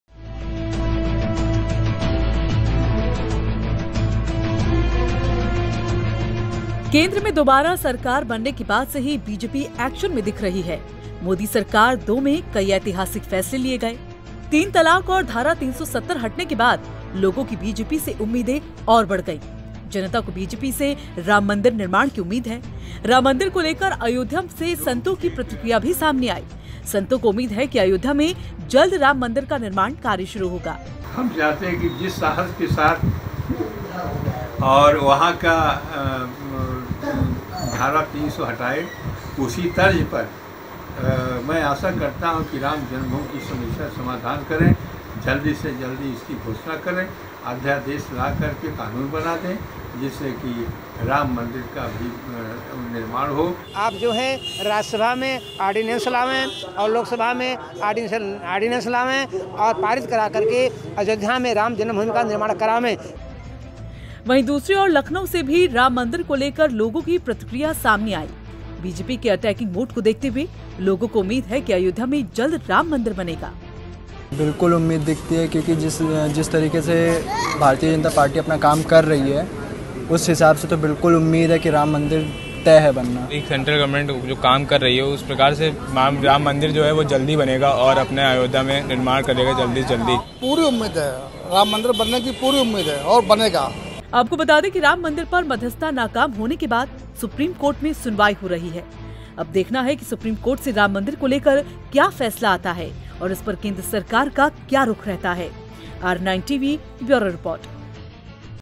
Article 370 खत्म, अब बनेगा राम मंदिर !
केंद्र में दोबारा पूर्ण बहुमत मिलने के बाद बीजेपी फ्रंट फुट पर है. बीजेपी एक के बाद एक ऐतिहासिक फैसले ले रही है...जिससे जनता की मोदी सरकार से उम्मीदें बढ़ गई हैं.